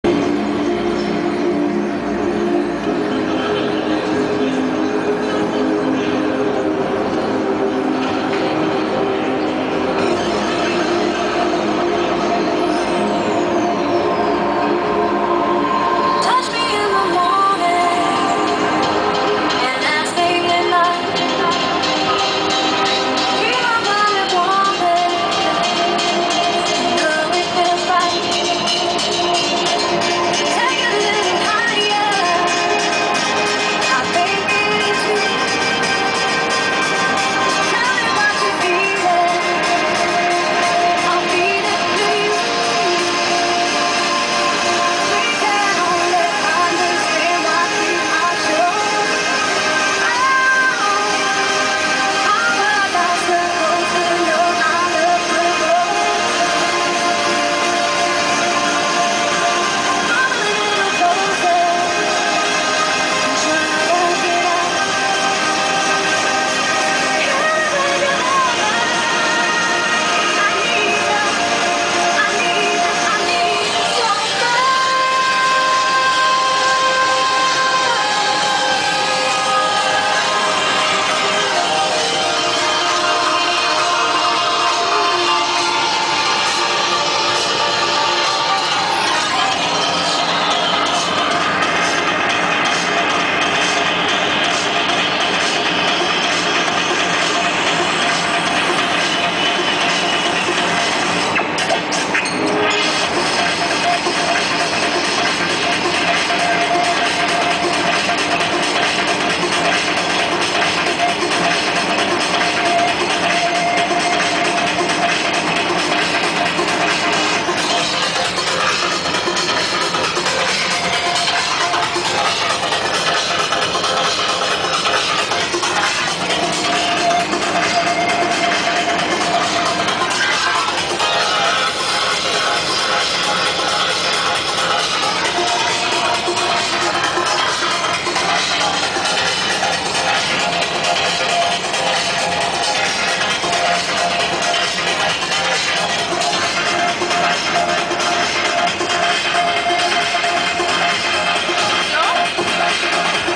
BMW 328 max speed
Tried to hit maximum speed at BMW 328 i